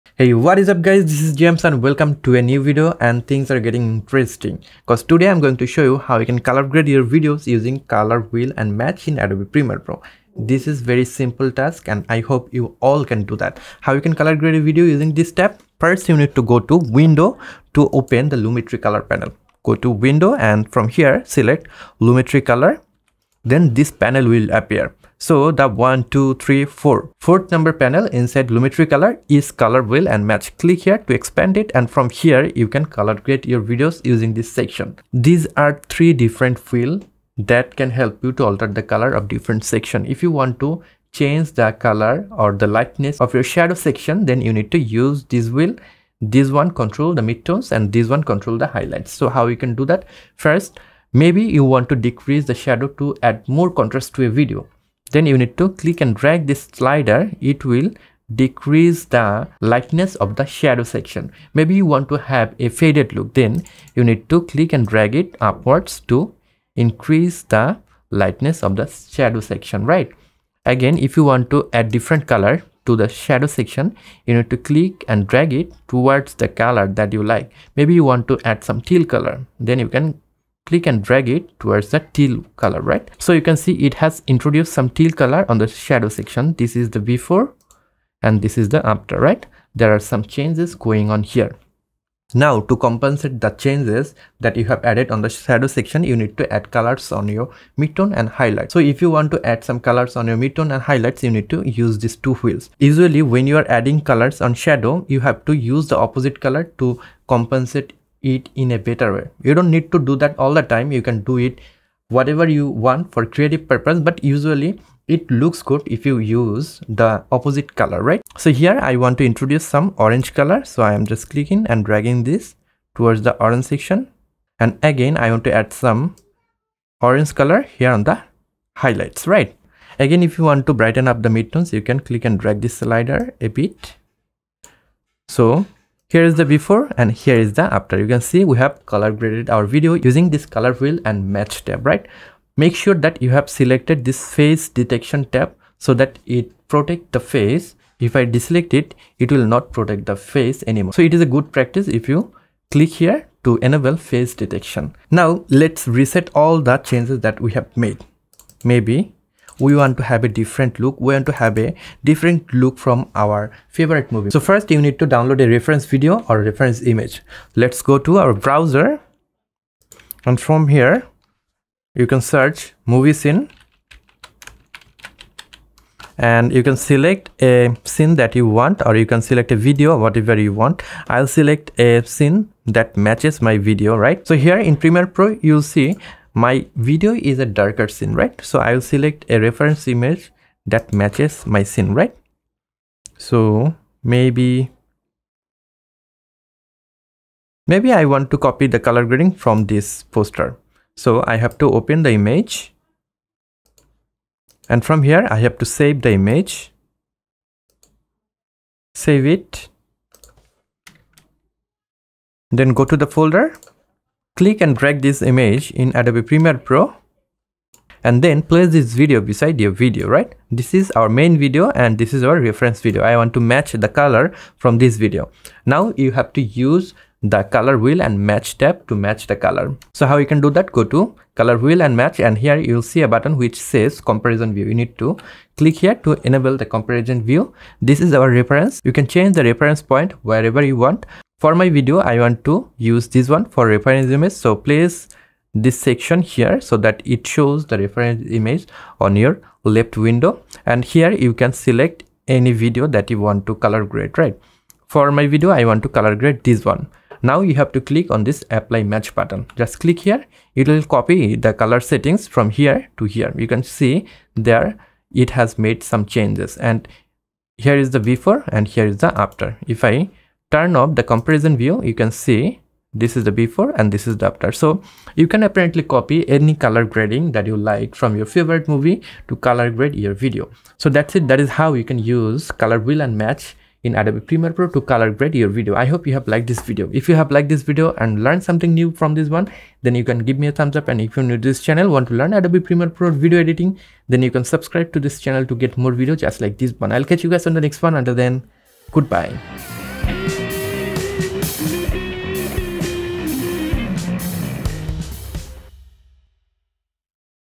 How to Use Color Wheel & Match in Premiere Pro | EP 35
Today I'll show you how you can use the color wheel & match in Adobe Premiere Pro to color grade your videos. Also, I'll show you how you can copy any color grading using the color wheel and match tab in Lumetri color.